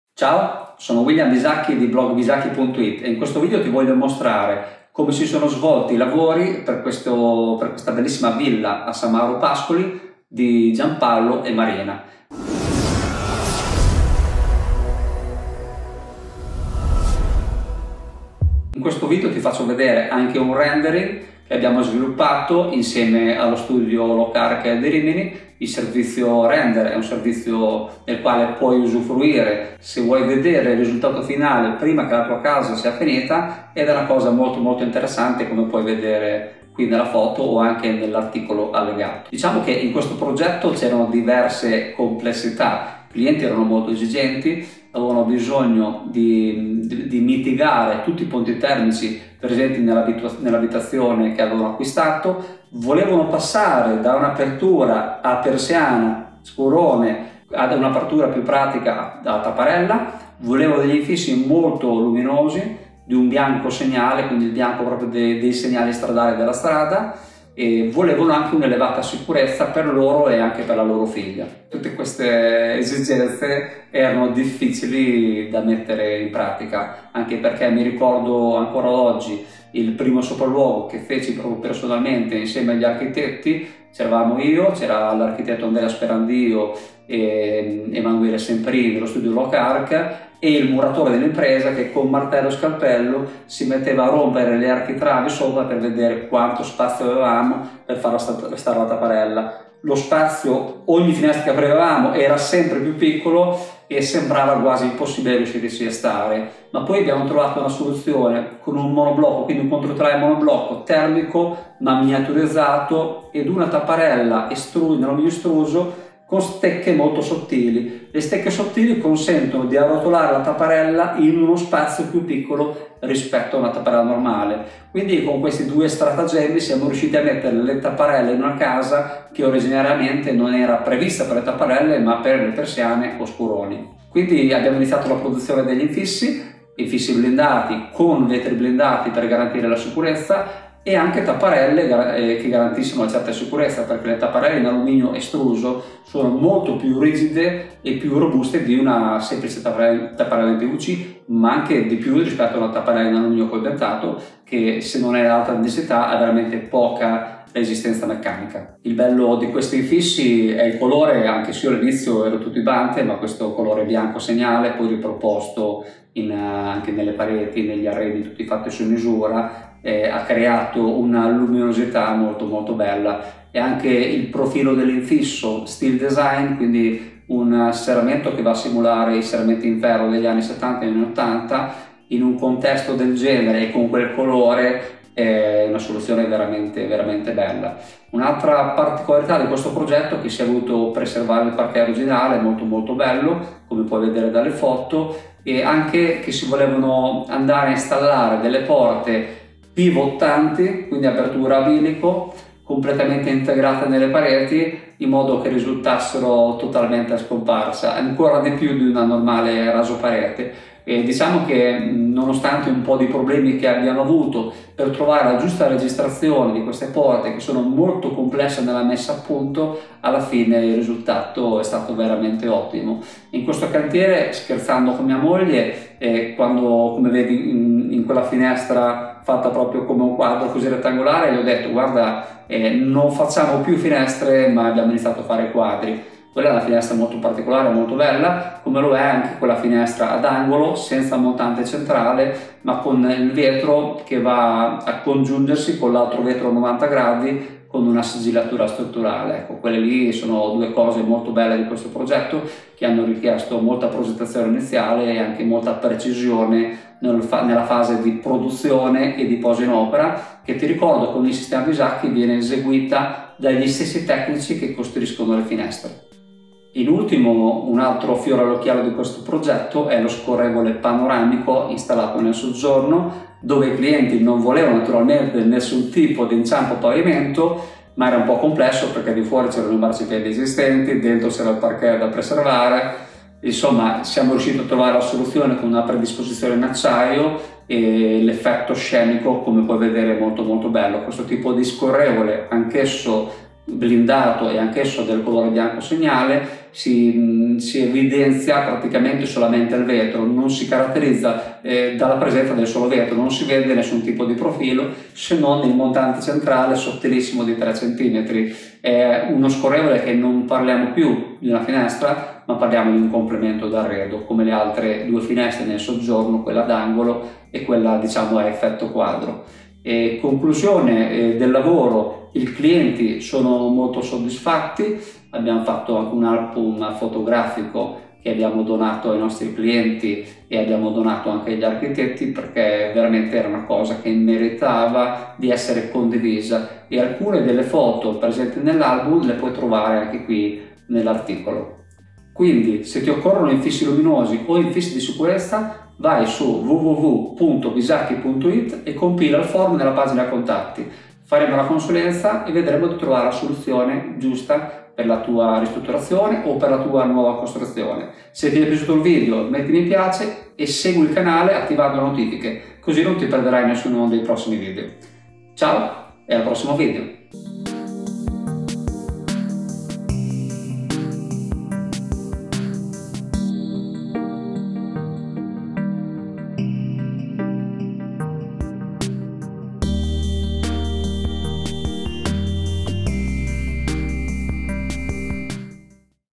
INFISSI IN ALLUMINIO VILLA A SAN MAURO PASCOLI
Infissi in alluminio San Mauro Pascoli per la villa di Gianpaolo e Marina

Era la primavera del 2017 quando Gianpaolo e Marina mi parlano dell’idea di ristrutturare la nuova casa a San Mauro Pascoli e dove, nel 2018, abbiamo installato i nostri infissi in alluminio slim.

Gianpaolo e Marina, già nostri clienti, si rivolgono a noi per questo nuovo progetto. Lo studio di architettura al quale si affidano è Locarc di Rimini, gli architetti sono: Andrea Sperandio, Alessandro Lontani e Emanuele Semprini.

Locarc è uno studio giovane, fondato nel 2014 ma i 3 architetti sono molto preparati, collaborativi ma decisi e presidiano il cantiere ogni giorno, questi aspetti sono stati molto apprezzati sia dai clienti che da noi.

▶️ Le esigenze che dovevamo a risolvere erano molteplici:

Alta sicurezza antieffrazione (per loro e loro figlia)
Luminosità degli ambienti
Finitura degli infissi con uno speciale colore bianco segnale
Eliminare i ponti termici dello scheletro in cemento armato
Sostituzione del sistema di oscuramento realizzato con scuroni con qualcosa di più pratico
Porte che si integrassero nelle pareti
Salvare il Parquet originale
Alcune di queste esigenze hanno generato problemi difficili da risolvere, io ed il mio ufficio tecnico insieme agli architetti abbiamo lavorato per diversi mesi prima di trovare le soluzioni migliori.

Prima di passare alla descrizione della ristrutturazione, lascia che ti mostri il Render e il risultato finale. Stupefacente vero? Il Render è un servizio premium che mettiamo a disposizione dei nostri clienti più esigenti in modo che possano vedere il risultato finale con un’elevata accuratezza.

Non è sempre facile per chi non è addetto ai lavori immaginarsi come sarà il risultato finale ed il nostro servizio Render aiutata proprio a visualizzare il risultato finale.

▶️ I problemi
Uno dei principali problemi che abbiamo incontrato è stato adattare una soluzione di oscuramento a tapparella avvolgibile in una costruzione che nasceva con i classici scuroni in legno.

Inoltre dovevamo risolvere tutti i ponti termici che si presentavano con i pilastri e le travi in cemento armato esistenti e naturalmente non modificabili.

In ultimo il parquet da salvaguardare e riuscire a fare coesistere con le porte a bilico e una grande vetrata scorrevole panoramica nel soggiorno.

▶️ Infissi in alluminio San Mauro Pascoli: Le soluzioni